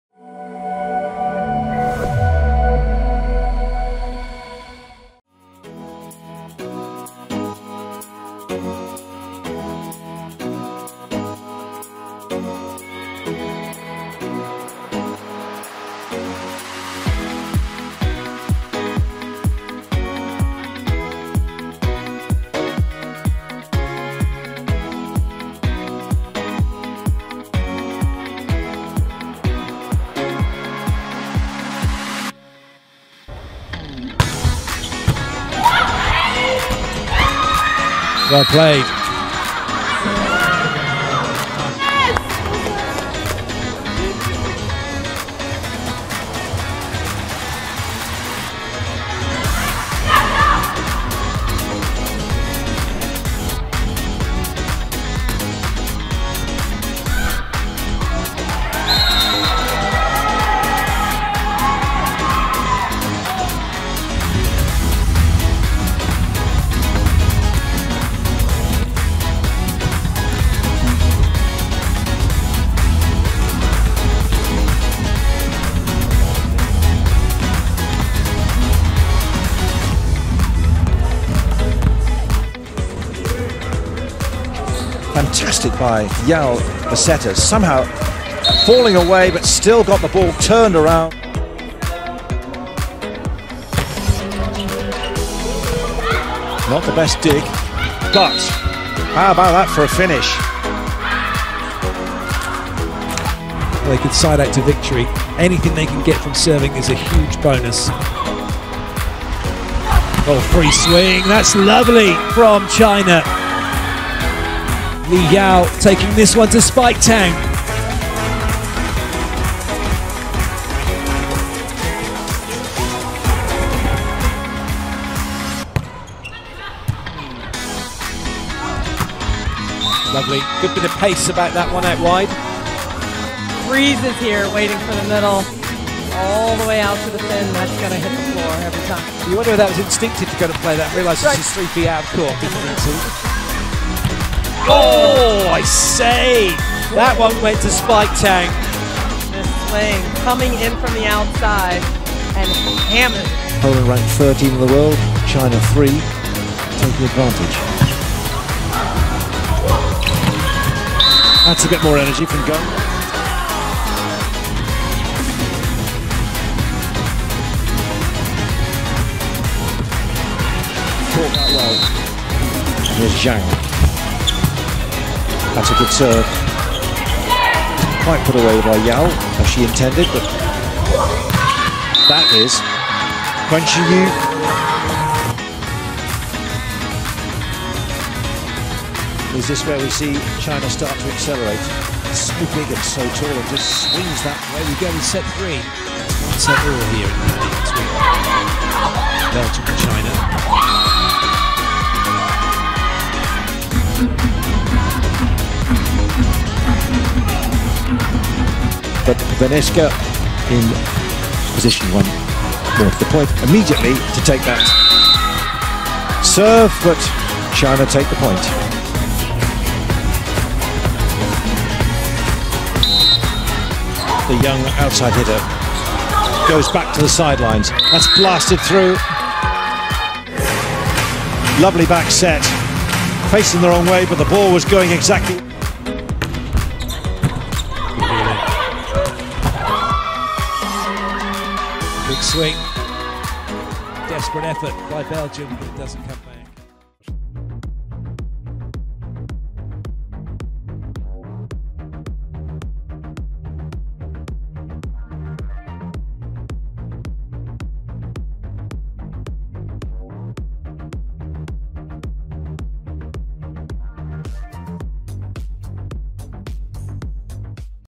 Di Yao (姚迪) Amazing Volleyball Setter from China | VNL 2021
Yao Di (姚迪; born 15 August 1992, in Tianjin), is a female Chinese volleyball player. She is the setter of China women's national volleyball team，and plays for Tianjin Bohai Bank.

Career
Yao won the 2013 FIVB U23 World Championship and named Most Valuable Player and best setter. She participated in the 2015 FIVB World Grand Prix. and 2019 Montreux Volley Masters,

Clubs
China Tianjin

Awards
Individuals
2011 U20 World Championship "Best Setter"
2013 U23 World Championship "Most Valuable Player"
2013 U23 World Championship "Best Setter"
2017 Asian Women's Club Volleyball Championship "Best Setter"